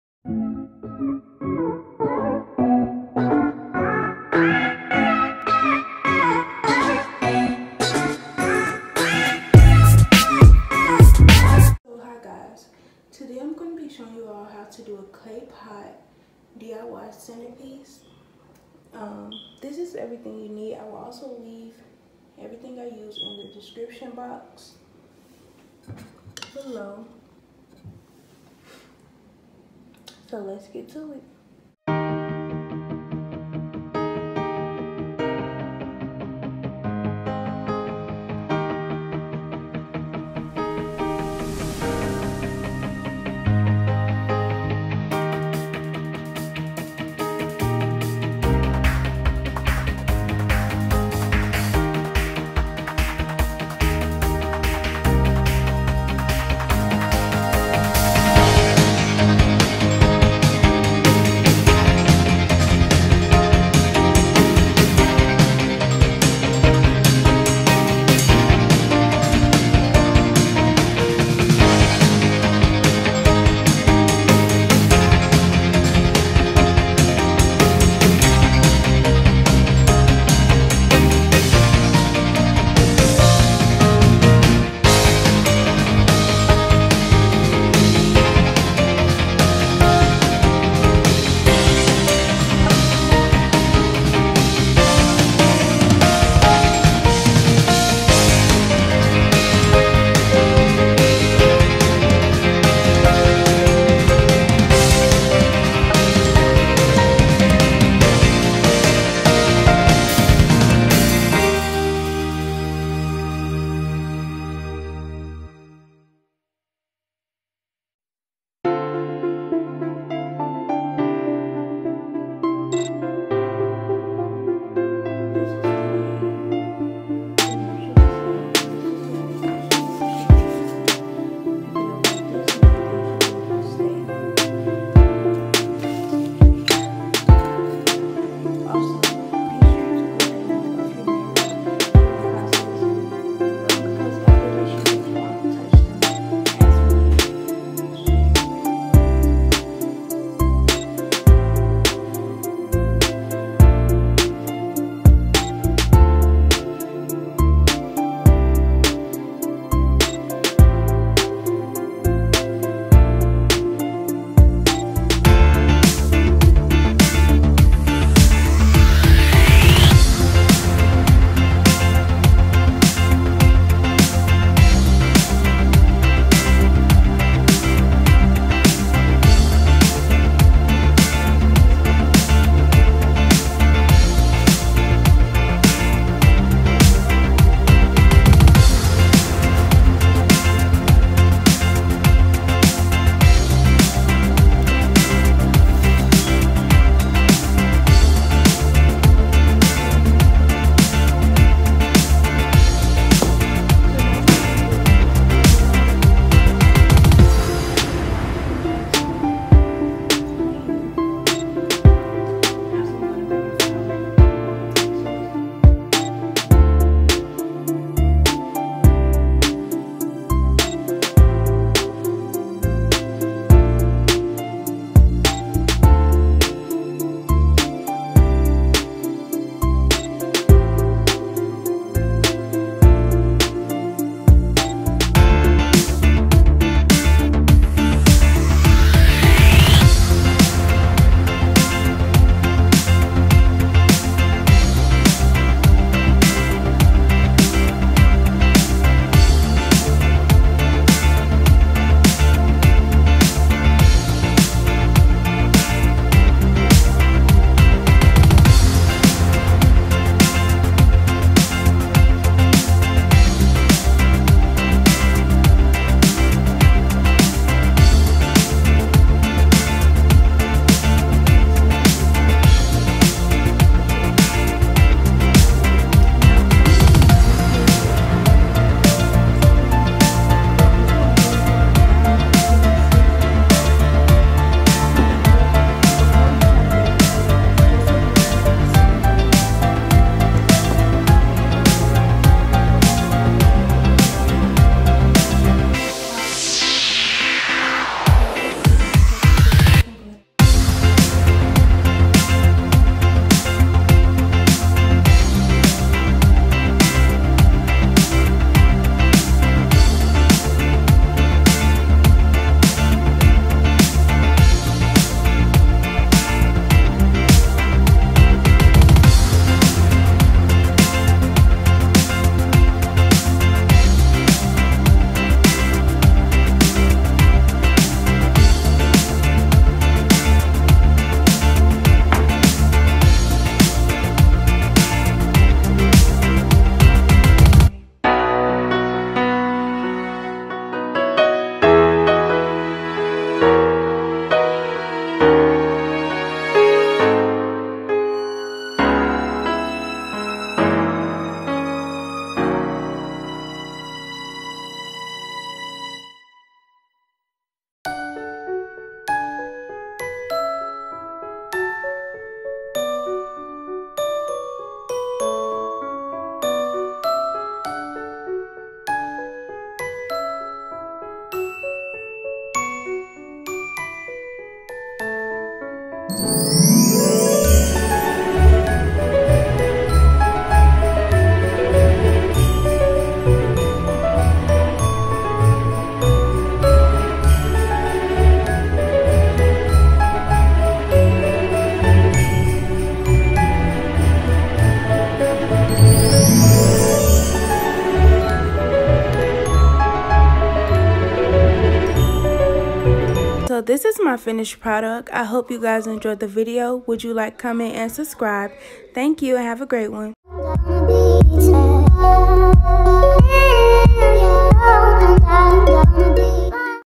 D.I.Y Clay Pot Centerpiece
Items Needed:
4 Medium Clay Pots
4 Small Square Mirriors
1 White Poster Board
2 Containers Of Black Paint
1 Tube Of Super Glue
1 Roll Of Rhinestones
1 Razor
A Large Pack Of Clear Rocks
2 Sets Of Flowers
1 Paint Brush
Glue Gun & Glue Sticks